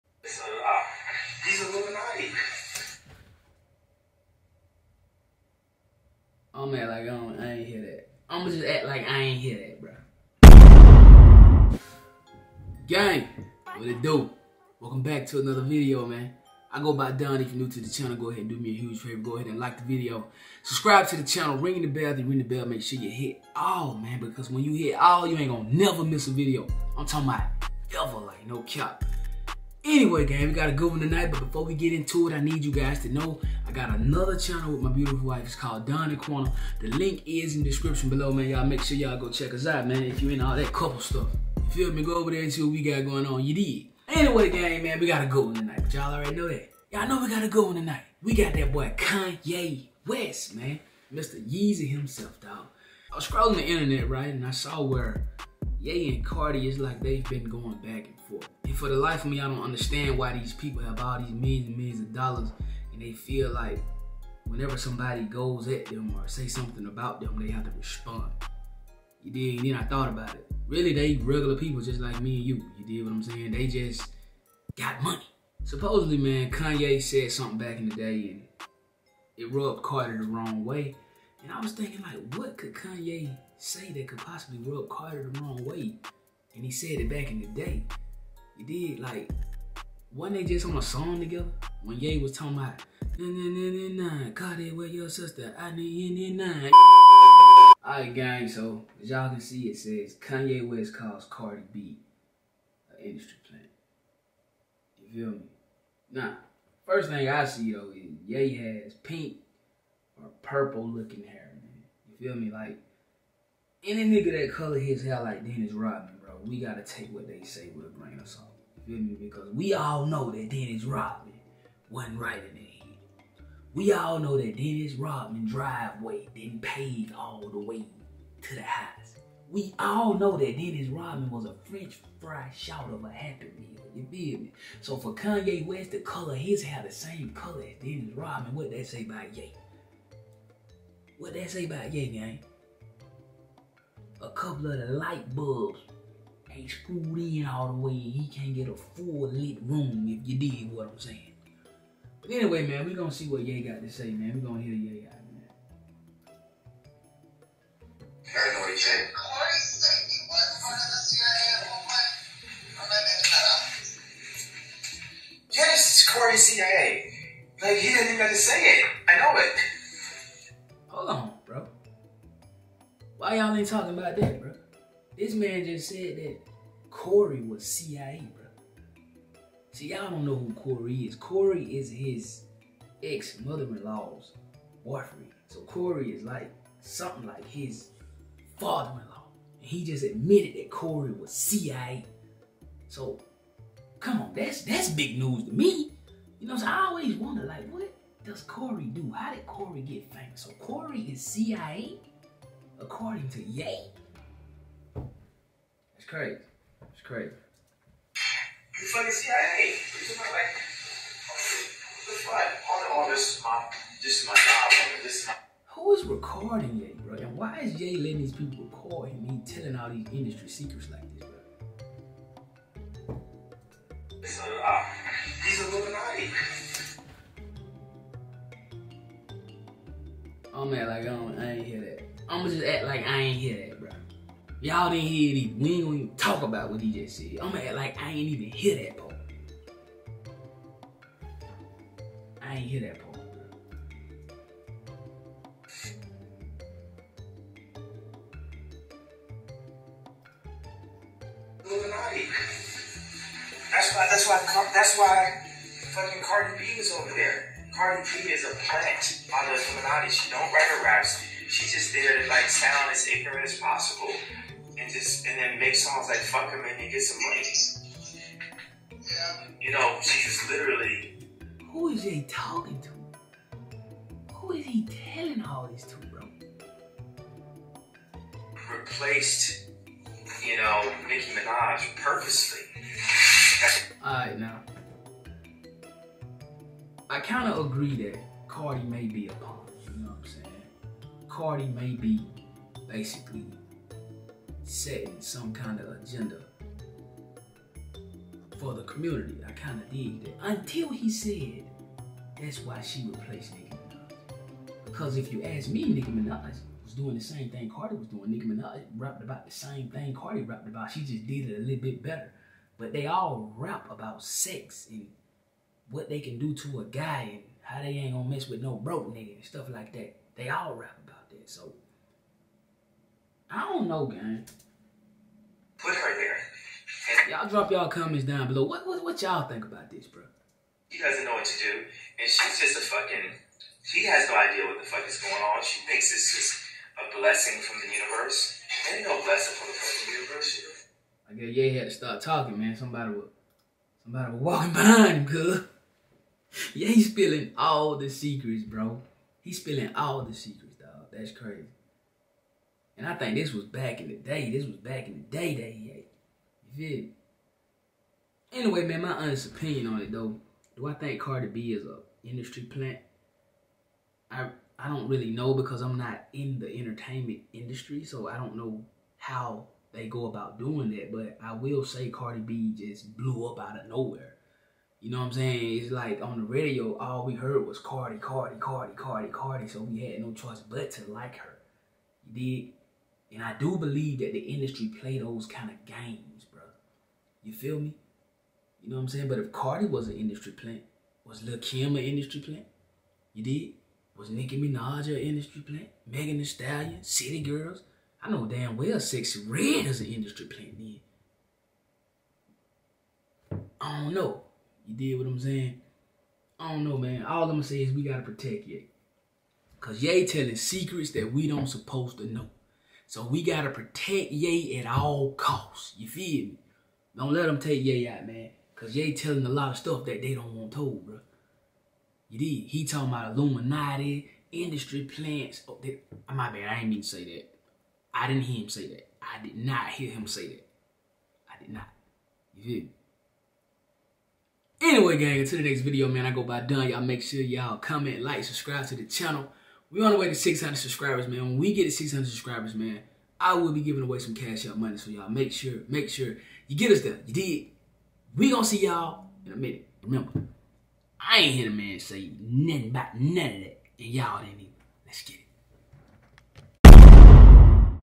KANYE EXPOSES THE INDUSTRY “ CARDI B IS JUST A PUPPET, BUT SHE THINK IT’S GOD “
GANG WHAT UP WE BACK WITH ANOTHER ONE AND TODAY WE GOT YE… KANYE DECIDED TO EXPOSE THE WHOLE INDUSTRY MAN… HE SAY CORY ILLUMINATI, JAY Z THIS, BEYONCÉ THAT, AND CARDI B DON’T KNOW WHAT THE HELL GOING ON 🤦🏾‍♂️

THIS IS MUST SEE TV‼️‼️‼️

Y’ALL TAP IN‼️‼️‼️